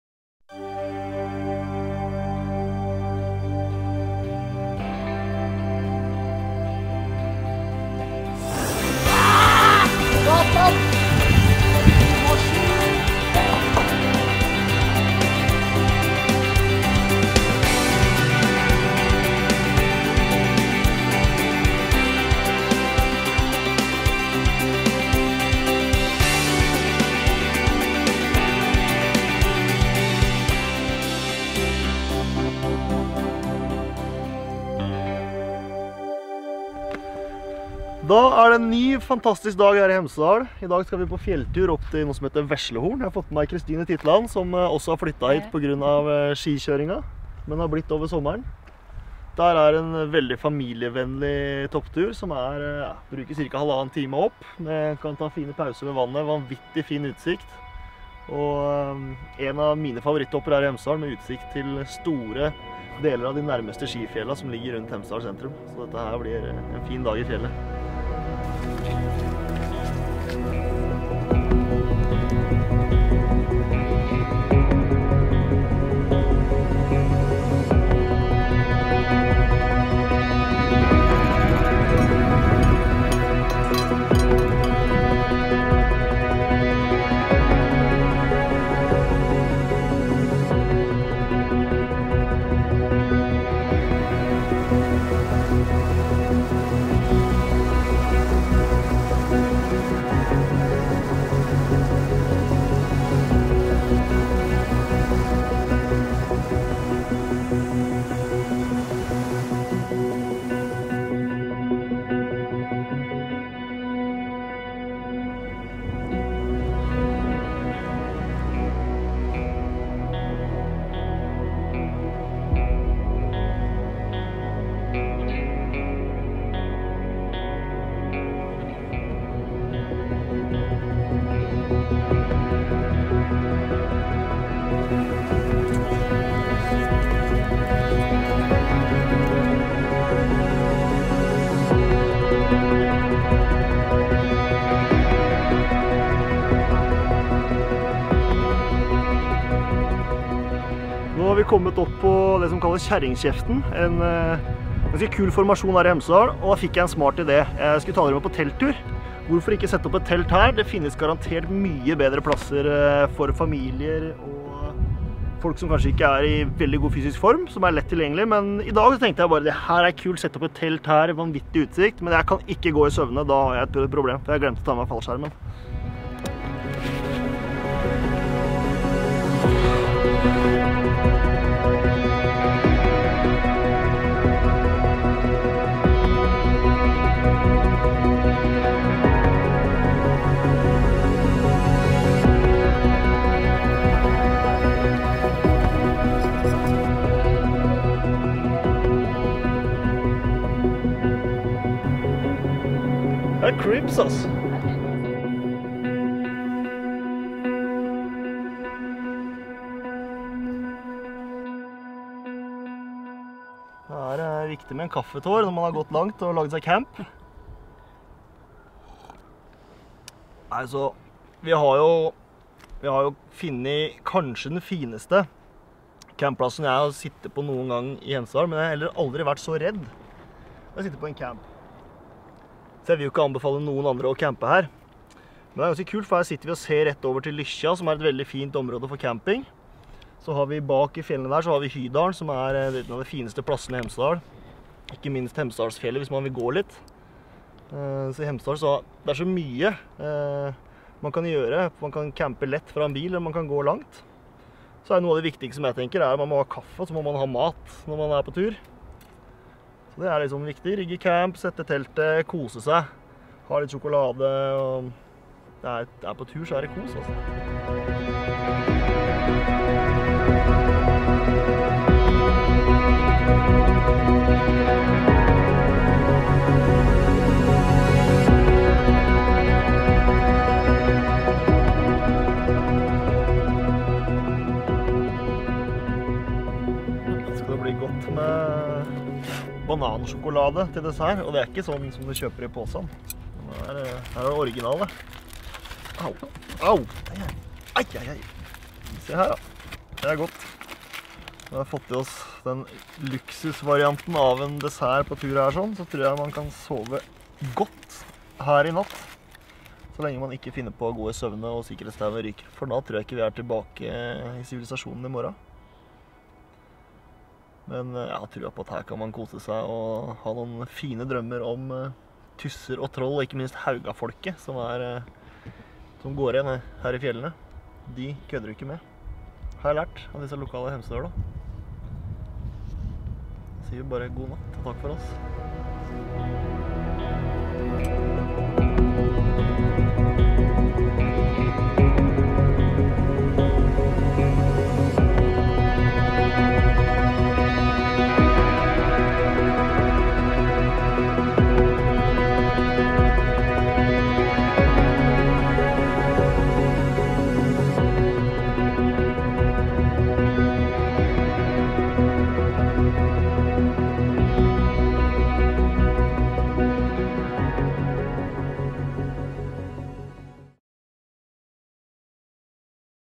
Hiking and camping in Hemsedal, Norway
Hiking in the Scandinavian Alps - Hemsedal is a magical experience. You have easy access to the high mountains and magnificent views. You can try the hiking program Top 20 or bring your map and compass and find your own adventure.
Movie made by FriFlyt